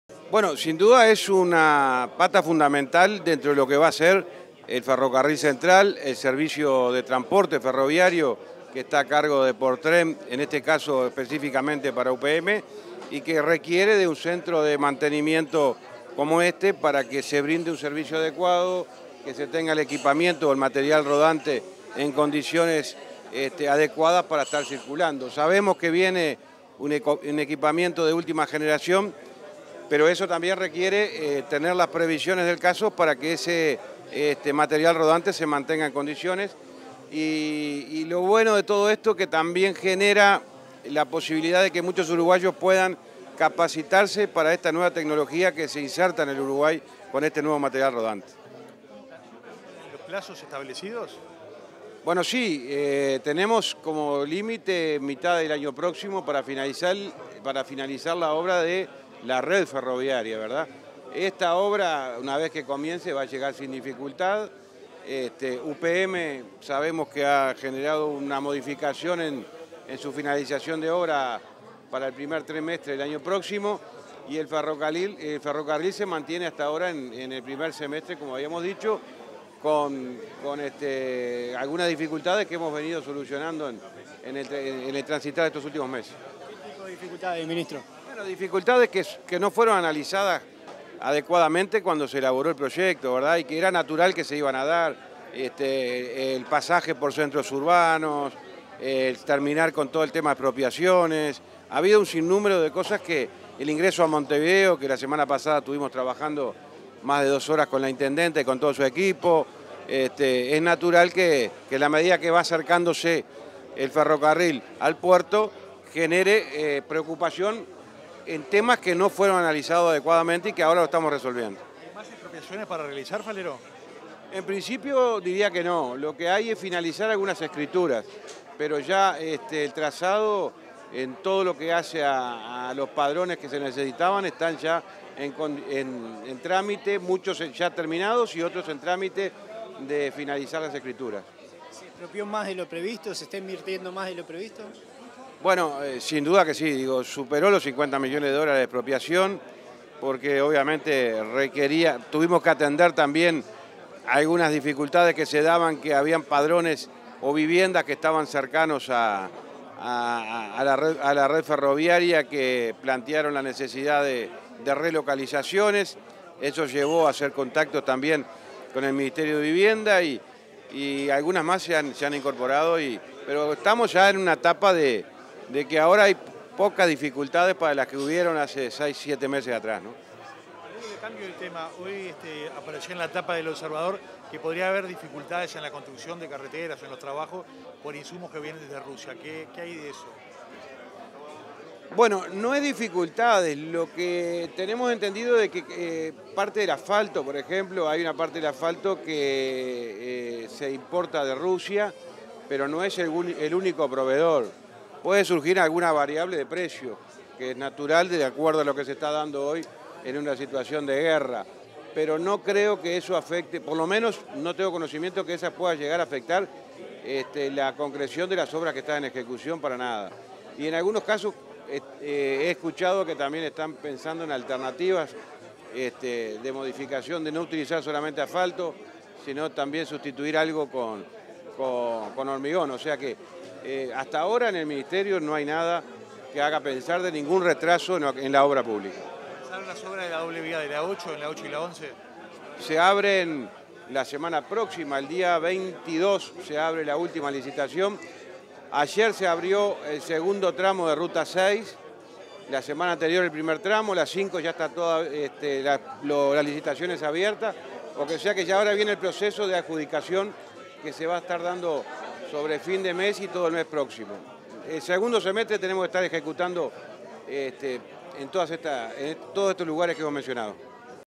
Declaraciones del ministro de Transporte y Obras Públicas, José Luis Falero
Tras participar de la presentación del inicio de obras del centro de operación y mantenimiento ubicado en ruta 5, en el departamento de Canelones, el ministro de Transporte y Obras Públicas, José Luis Falero, realizó declaraciones a la prensa.